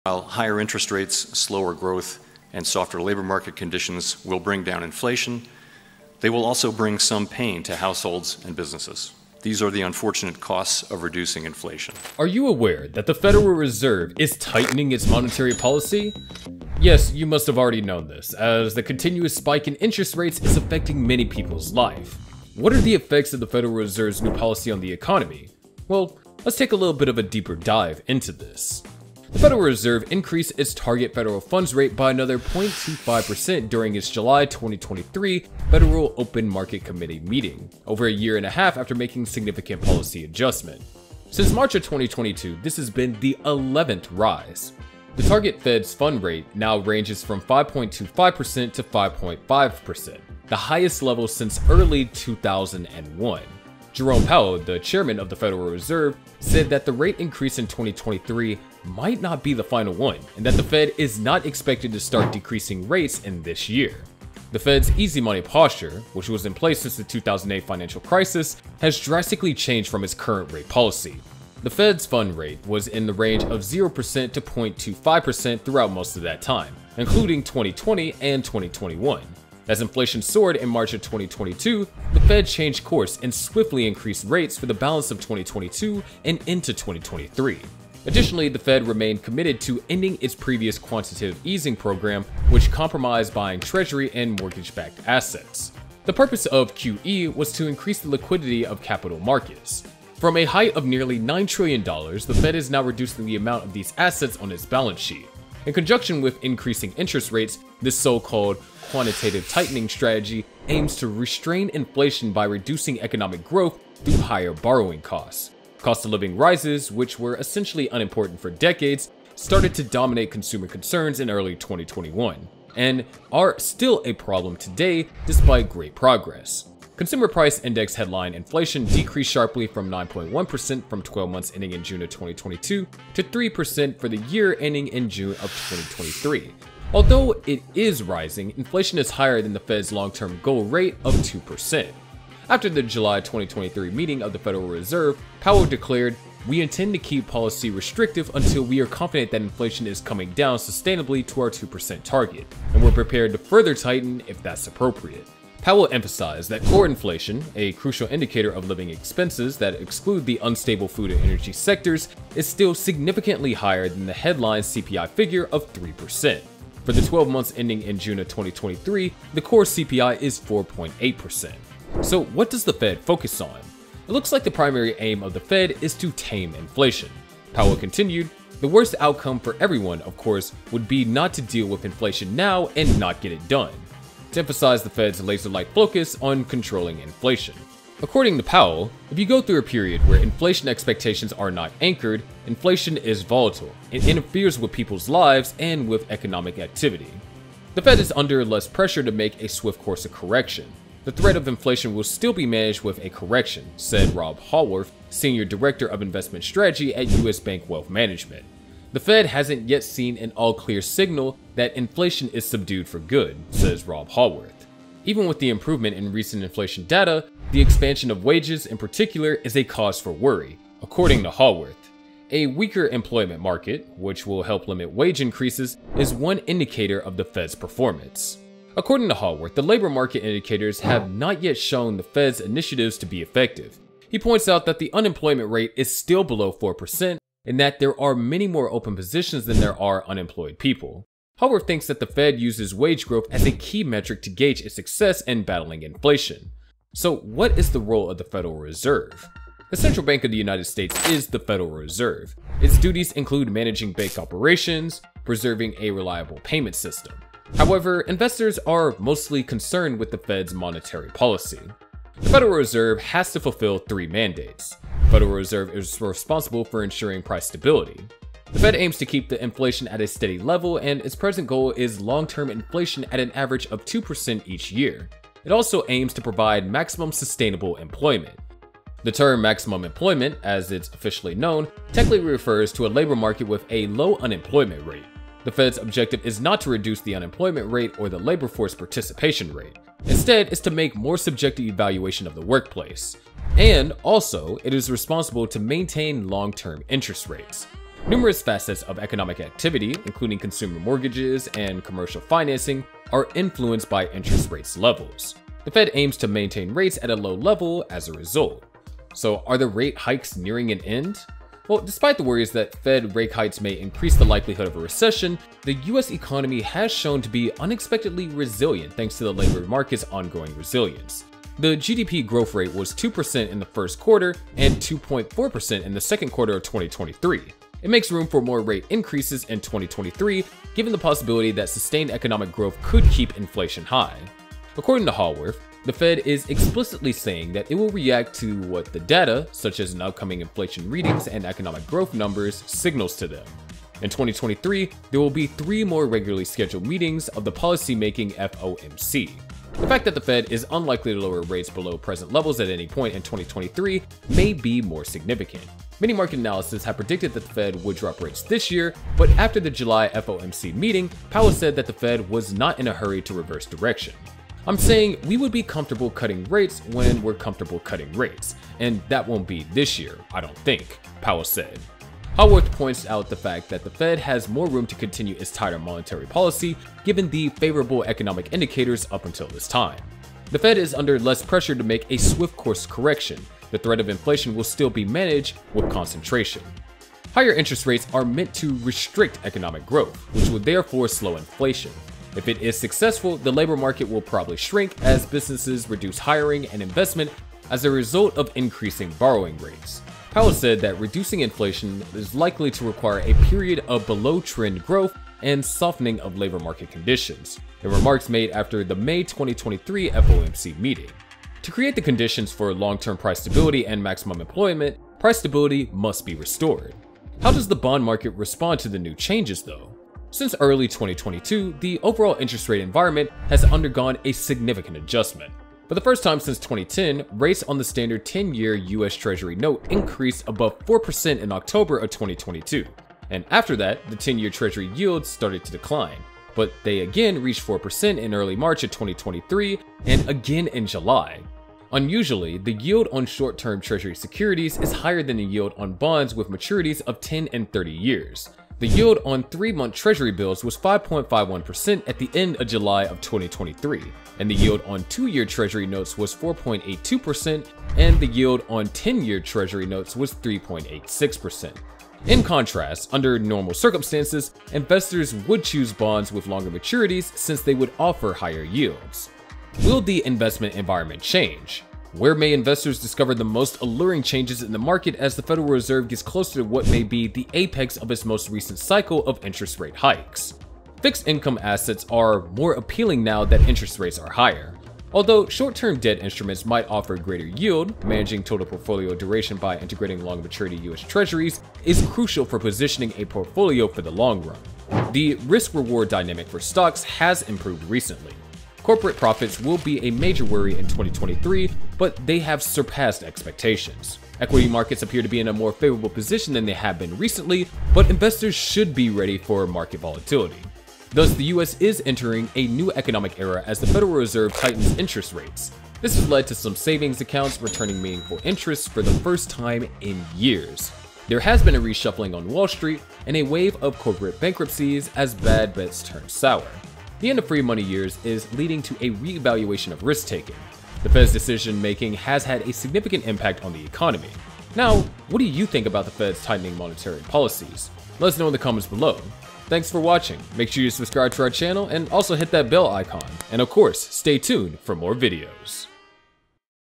How The Federal Reserve's Decision Making Has Affected The Economy
Do you know that the Federal Reserve is making money more expensive? Yes, you probably already knew this, since the constant rise in interest rates is making a lot of people's lives harder. What changes do the new strategies of the Federal Reserve make to the economy? Let's look into this a little more.